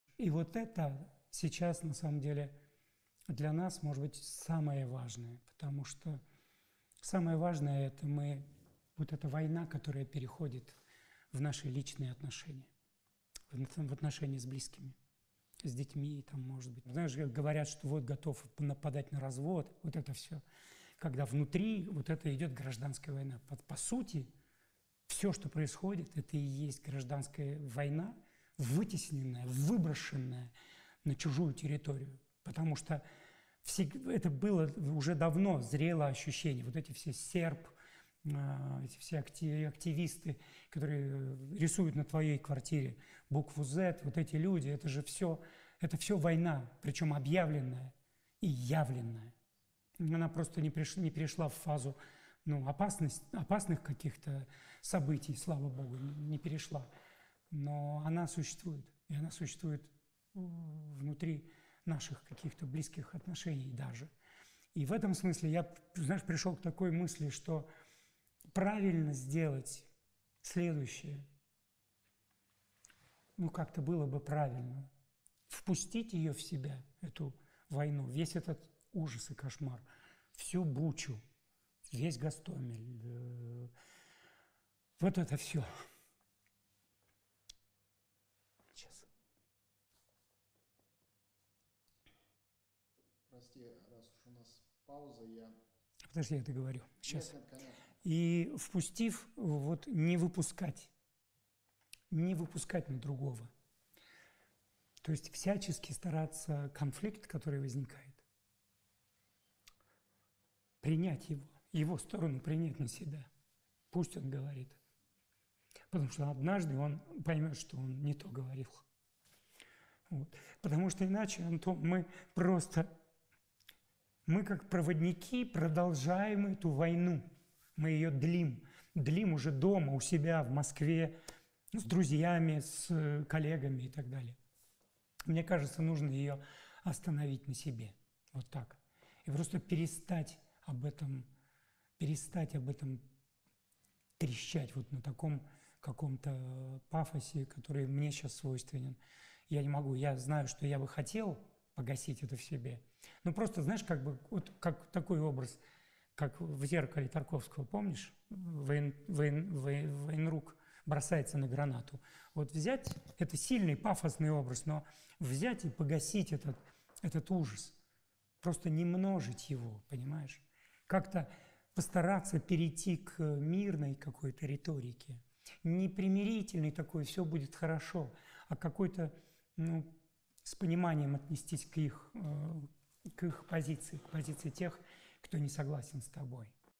Звягинцев - цитата из виедоинтервью Антону Долину
Все что происходит это и есть гражданская война, вытесненная, выброшенная на чужую территорию. Это было уже давно. Зрело ощущение. Вот эти все SERB, и все эти активисты, которые пишут на твоей квартире буквы зет, вот эти люди, это же всё война. Причем объявленная и явленная. Она просто не перешла в фазу опасных каких-то событий. Слава Б-гу не перешла. Но она существует. Она существует внутри наших каких-то близких отношений даже.
И в этом смысле я пришел к такой мысли, что правильно сделать следующее. Ну как-то было бы правильно. Впустить её в себя эту войну. Весь этот ужас и кошмар. Всю Бучу. Весь Гостомель. Вот это всё.
И впустив вот не выпускать не выпускать другого. То есть всячески стараться конфликт, который возникает, принять его. Его сторону принять на себя. Пусть он говорит. Потому что однажды он поймёт что он не то говорил. Потому что иначе мы просто как проводники продолжаем эту войну. Мы ее длим.  Длим уже дома, у себя, в Москве, с друзьями, с коллегами и так далее.
Мне кажется нужно её остановить на себе. Вот так.
И просто перестать об этом трещать вот на таком каком-то пафосе, который и мне сейчас свойственен. Я не могу. Я знаю, что я бы хотел погасить это в себе. Знаешь как в Зеркале Тарковского, помнишь, военрук бросается на гранату. Это сильный пафосный образ, но взять и погасить этот ужас. Просто не множить его, понимаешь.